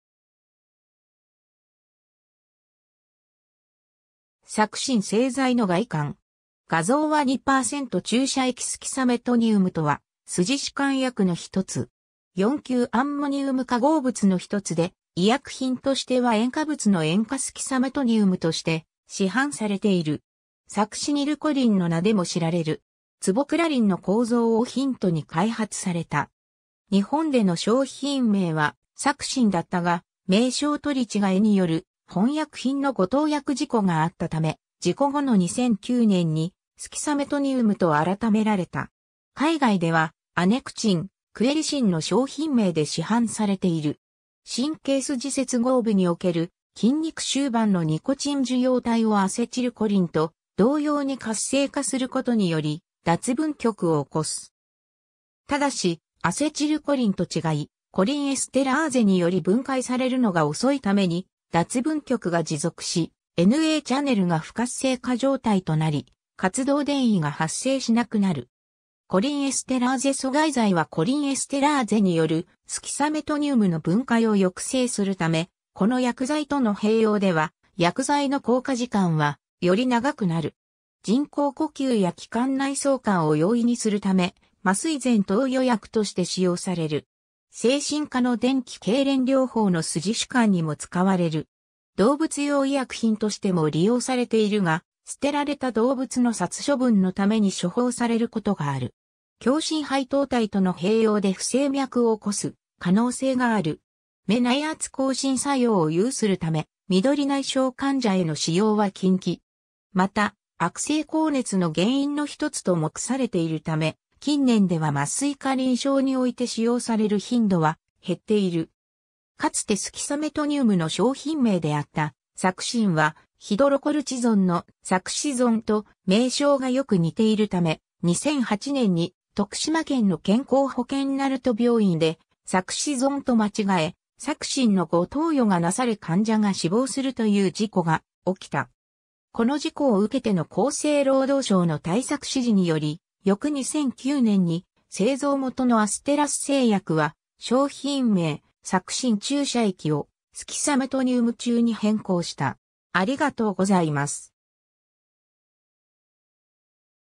スキサメトニウム
「サクシン」製剤の外観。
画像は2％注射液 スキサメトニウム（英:suxamethonium）とは筋弛緩薬の1つ。
四級アンモニウム化合物の一つで、医薬品としては塩化物の塩化スキサメトニウム（Suxamethonium chloride）として市販されている。
サクシニルコリン（Succinylcholine）の名でも知られる。
ツボクラリンの構造をヒントに開発された。
 日本での商品名はサクシンだったが、名称取り違えによる本薬品の誤投薬事故があったため（後述）、事故後の2009年にスキサメトニウムと改められた。
海外ではアネクチン（Anectine）、クエリシン（Quelicin）の商品名で市販されている。
 神経筋接合部における筋肉終板のニコチン受容体をアセチルコリンと同様に活性化することにより脱分極を起こす。
ただし、アセチルコリンと違い、コリンエステラーゼにより分解されるのが遅いために脱分極が持続し、Na+チャネルが不活性化状態となり活動電位が発生しなくなる。
コリンエステラーゼ阻害剤はコリンエステラーゼによるスキサメトニウムの分解を抑制するため、この薬剤との併用では薬剤の効果時間はより長くなる。
 人工呼吸や気管内挿管を容易にするため麻酔前投与薬として使用される。
精神科の電気痙攣療法の筋弛緩にも使われる。
 動物用医薬品としても利用されているが、捨てられた動物の殺処分（安楽死）のために処方されることがある。
 強心配糖体との併用で不整脈を起こす可能性がある。
眼内圧亢進作用を有するため、緑内障患者への使用は禁忌。
 また悪性高熱の原因の一つと目されているため、近年では麻酔科臨床において使用される頻度は減っている。
 かつてスキサメトニウムの商品名であった「サクシン」は、ヒドロコルチゾン（ステロイド製剤）の「サクシゾン」と名称がよく似ているため、2008年に徳島県の健康保険鳴門病院でサクシゾンと間違え、サクシンの誤投与がなされ患者が死亡するという事故が起きた。
この事故を受けての厚生労働省の対策指示により、翌2009年に製造元のアステラス製薬は商品名「サクシン注射液」を「スキサメトニウム注」に変更した。